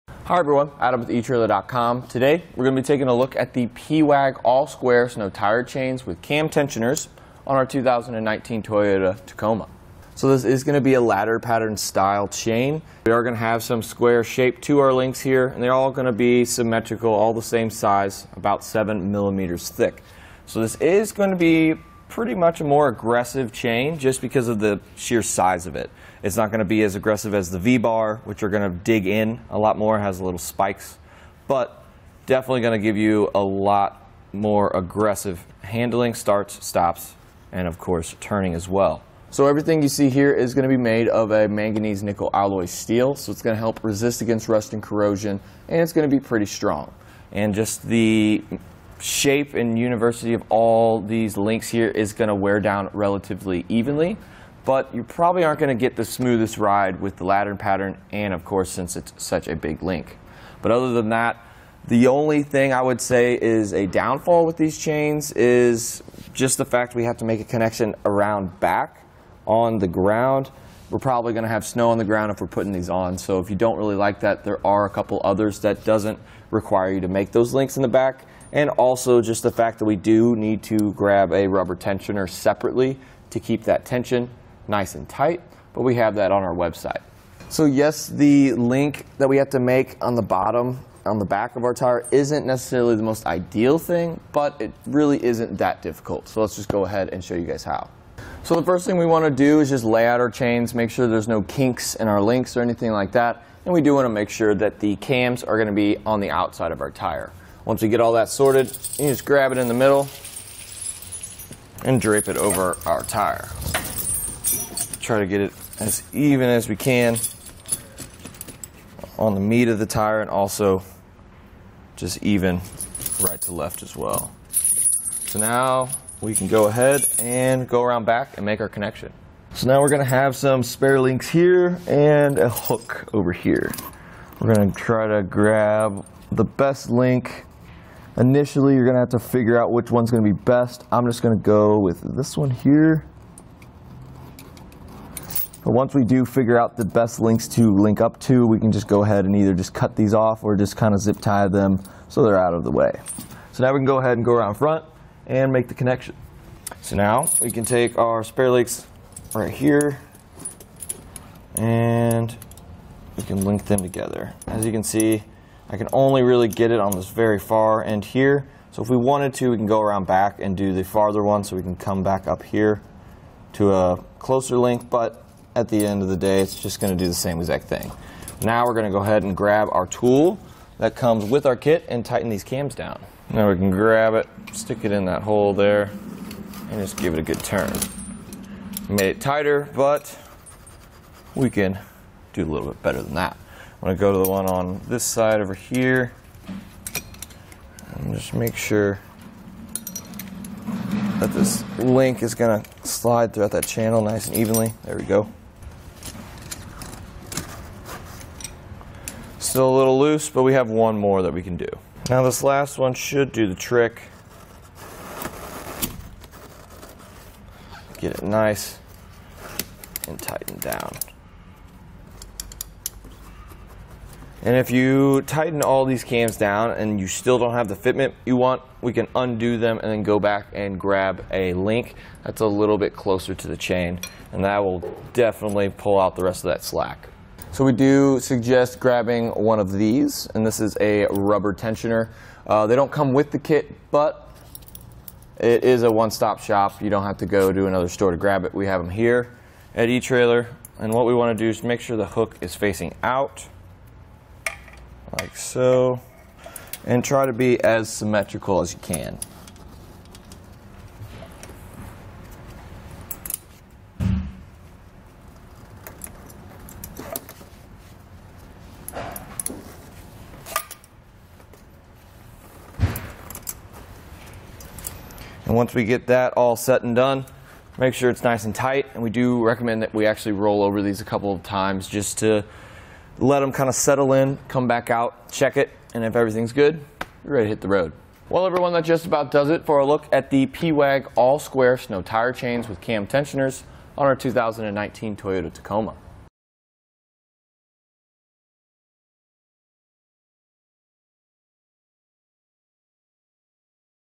etrailer | Setting up the pewag Wide Base Tire Chains w Cams on your 2019 Toyota Tacoma
Adam:                                                  Hi, everyone. Adam with etrailer.com. Today we're going to be taking a look at the Pewag All Square Snow Tire Chains with cam tensioners on our 2019 Toyota Tacoma. So this is going to be a ladder pattern style chain. We are going to have some square shape to our links here, and they're all going to be symmetrical, all the same size, about seven millimeters thick. So this is going to be pretty much a more aggressive chain, just because of the sheer size of it.

It's not going to be as aggressive as the V-Bar, which are going to dig in a lot more, has little spikes, but definitely going to give you a lot more aggressive handling, starts, stops, and of course turning as well. So everything you see here is going to be made of a manganese nickel alloy steel. So it's going to help resist against rust and corrosion, and it's going to be pretty strong. And just the shape and university of all these links here is going to wear down relatively evenly, but you probably aren't going to get the smoothest ride with the ladder pattern. And of course, since it's such a big link.But other than that, the only thing I would say is a downfall with these chains is just the fact that we have to make a connection around back on the ground.

We're probably going to have snow on the ground if we're putting these on. So if you don't really like that, there are a couple others that doesn't require you to make those links in the back. And also just the fact that we do need to grab a rubber tensioner separately to keep that tension nice and tight, but we have that on our website.So yes, the link that we have to make on the bottom on the back of our tire isn't necessarily the most ideal thing, but it really isn't that difficult. So let's just go ahead and show you guys how. So the first thing we want to do is just lay out our chains, make sure there's no kinks in our links or anything like that.

And we do want to make sure that the cams are going to be on the outside of our tire. Once you get all that sorted, you can just grab it in the middle and drape it over our tire. Try to get it as even as we can on the meat of the tire and also just even right to the left as well. So now we can go ahead and go around back and make our connection.So now we're going to have some spare links here and a hook over here. We're going to try to grab the best link.

Initially you're going to have to figure out which one's going to be best. I'm just going to go with this one here. Once we do figure out the best links to link up to, we can just go ahead and either just cut these off or just zip tie them so they're out of the way. So now we can go ahead and go around front and make the connection. So now we can take our spare links right here and we can link them together. As you can see, I can only really get it on this very far end here. So if we wanted to, we can go around back and do the farther one so we can come back up here to a closer link. But at the end of the day, it's just going to do the same exact thing.Now we're going to go and grab our tool that comes with our kit and tighten these cams down. Now we can grab it, stick it in that hole there and just give it a good turn. Made it tighter, but we can do a little bit better than that. I want to go to the one on this side over here. Let me just make sure that this link is going to slide throughout that channel nice and evenly. There we go. Still a little loose, but we have one more that we can do. Now this last one should do the trick, get it nice and tightened down. And if you tighten all these cams down and you still don't have the fit you want, we can undo them and then go back and grab a link that's a little bit closer to the chain and that will definitely pull out the rest of that slack.So we do suggest grabbing one of these, and this is a rubber tensioner. They don't come with the kit, but it is a one-stop shop. You don't have to go to another store to grab it. We have them here at etrailer. And what we want to do is make sure the hook is facing out like so, and try to be as symmetrical as you can. And once we get that all set and done, make sure it's nice and tight. And we do recommend that we actually roll over these a couple of times, just to let them settle in, come back out, check it. And if everything's good, you're ready to hit the road.Well everyone not just about does it for a look at the Pewa